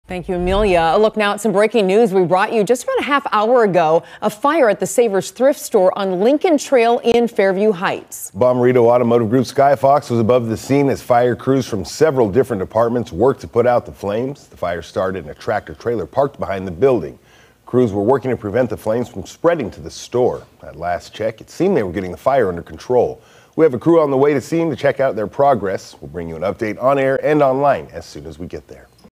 Massive fire near 'Savers' thrift store in Fairview Heights
A tractor trailer parked right outside 'Savers' thrift store on Lincoln Trail caught fire just about an hour ago.